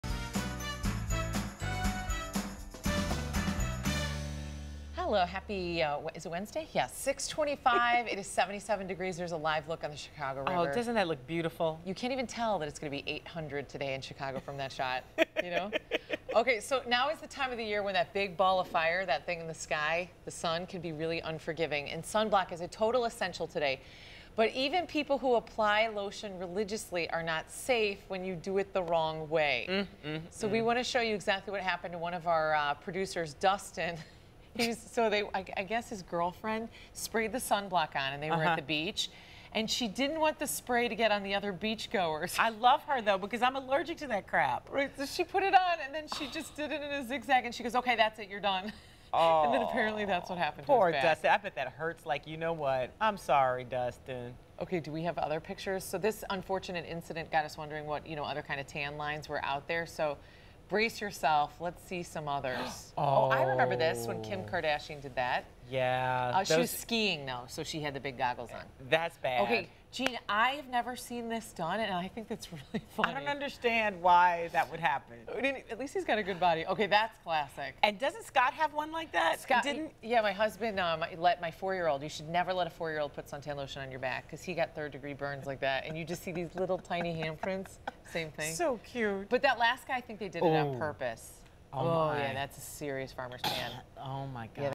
Sunburn
My wicked sunburn made local Chicago TV